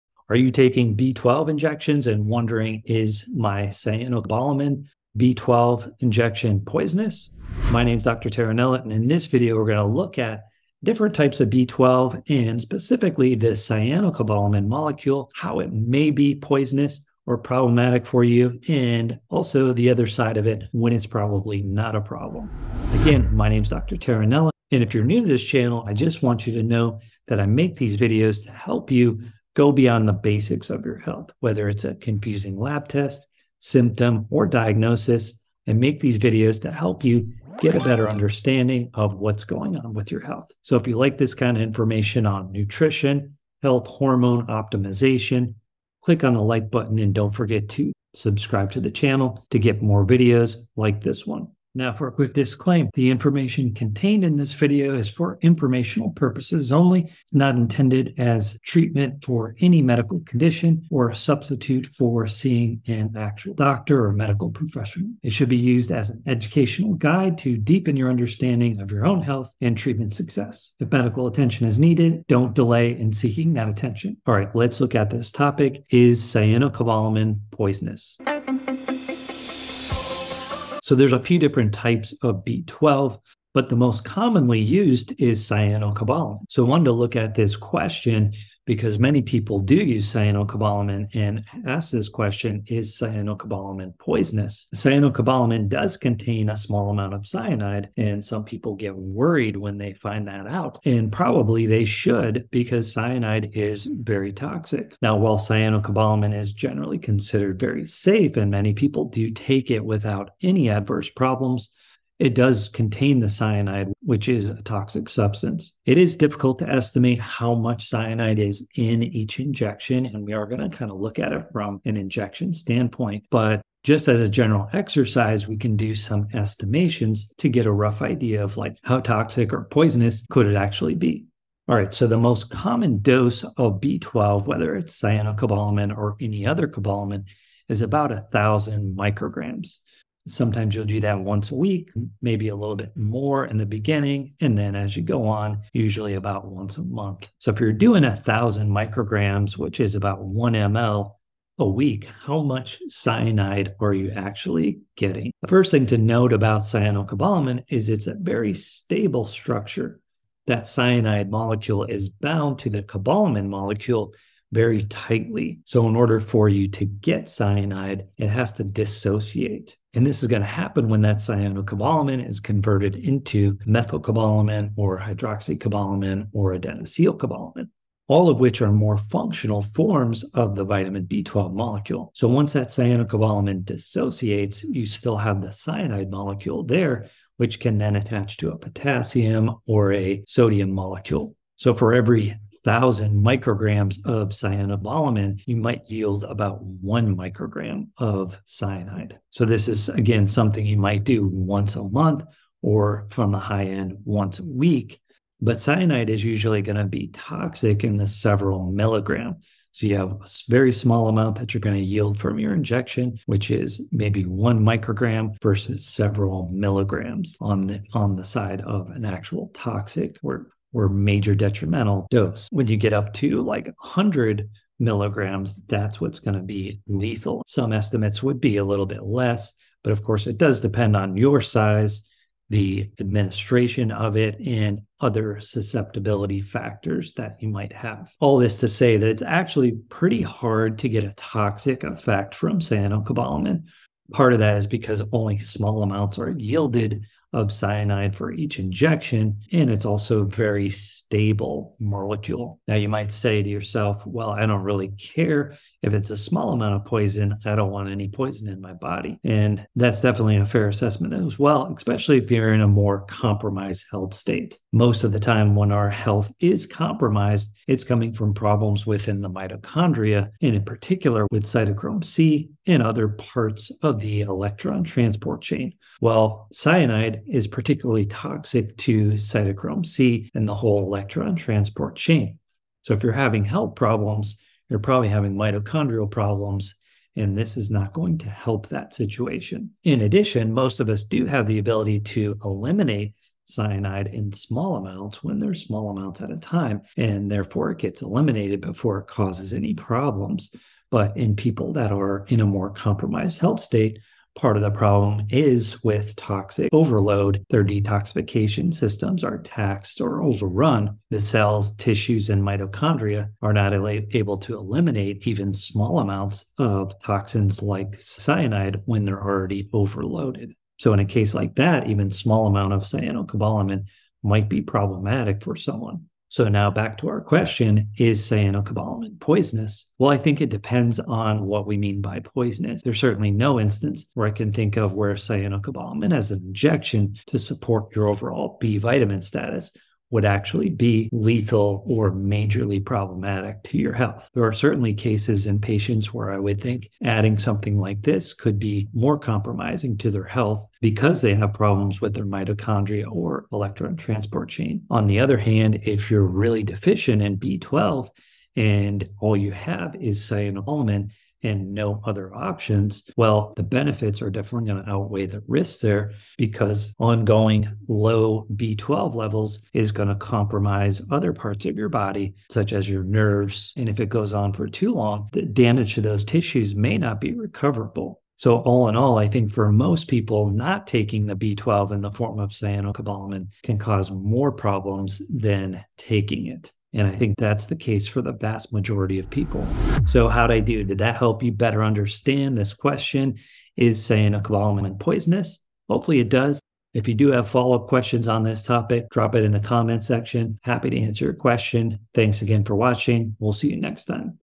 Is Cyanocobalamin Poisonous?
Are you taking B12 injections and wondering if your cyanocobalamin B12 injection is poisonous? My name is Dr. Terranella, and in this video, we're going to look at different types of B12 and specifically the cyanocobalamin molecule, how it may be poisonous or problematic for you, and also the other side of it, when it's probably not a problem.
So, there are a few different types of B12.

But the most commonly used is cyanocobalamin.  So, I wanted to look into this question because many people do use cyanocobalamin and ask if it is poisonous. Cyanocobalamin does contain a small amount of cyanide, and some people get worried when they find that out. And probably they should because cyanide is very toxic.  Now, while cyanocobalamin is generally considered very safe, and many people do take it without any adverse problems, it does contain cyanide, which is a toxic substance. It is difficult to estimate how much cyanide is in each injection, but we can do some estimations to get a rough idea of how toxic or poisonous it could actually be.

All right, so the most common dose of B12, whether it's cyanocobalamin or any other cobalamin, is about 1,000 micrograms. Sometimes you'll do that once a week, maybe a little bit more in the beginning, and then as you go on, usually about once a month.  So, if you're doing 1,000 micrograms, which is about 1 mL a week, how much cyanide are you actually getting?

First thing to note about cyanocobalamin is that it is a very stable structure. The cyanide molecule is bound to the cobalamin molecule very tightly. So, in order for you to get cyanide, it has to dissociate.

This will happen when that cyanocobalamin is converted into methylcobalamin, hydroxycobalamin, or adensylcobalamin, all of which are more functional forms of the vitamin B12 molecule. So, once that cyanocobalamin dissociates, you still have the cyanide molecule there, which can then attach to a potassium or a sodium molecule.

So, for every 1,000 micrograms of cyanocobalamin, you might yield about 1 microgram of cyanide. This is, again, something you might do once a month or on the high end, once a week. But cyanide is usually toxic in the several milligrams. So, you have a very small amount that you're going to yield from your injection, which is maybe 1 microgram versus several milligrams on the side of an actual toxic or major detrimental effect. If you get up to like 100 milligrams, that's what's going to be lethal.

Some estimates would be a little bit less, but of course it does depend on your size, the administration of it, and other susceptibility factors that you might have.

All this to say that it's actually pretty hard to get a toxic effect from cyanocobalamin. Part of that is because only small amounts are yielded of cyanide for each injection, and it's also a very stable molecule.  Now, you might say to yourself, "Well, I don't really care if it's a small amount of poison. I don't want any poison in my body," and that's definitely a fair assessment as well, especially if you're in a more compromised health state.  Most of the time when our health is compromised, it's coming from problems within the mitochondria. In particular, with cytochrome C and other parts of the electron transport chain. Well, cyanide is particularly toxic to cytochrome C and the whole electron transport chain.
So if you're having health problems, you're probably having mitochondrial problems, and this is not going to help that situation. In addition, most of us do have the ability to eliminate cyanide in small amounts when there's small amounts at a time, and therefore it gets eliminated before it causes any problems.

But in people that are in a more compromised health state, part of the problem is with toxic overload. detoxification systems are taxed or overrun.  The cells, tissues and mitochondria are not able to eliminate even small amounts of toxins like cyanide when they're already overloaded.

So in a case like that, even small amount of cyanocobalamin might be problematic for someone. So now back to our question, Is cyanocobalamin poisonous. Well, I think it depends on what we mean by poisonous. There's certainly no instance where I can think of where cyanocobalamin has an injection to support your overall B vitamin status would actually be lethal or majorly problematic to your health.  There are certainly cases in patients where I would think adding something like this could be more compromising to their health because they have problems with their mitochondria or electron transport chain.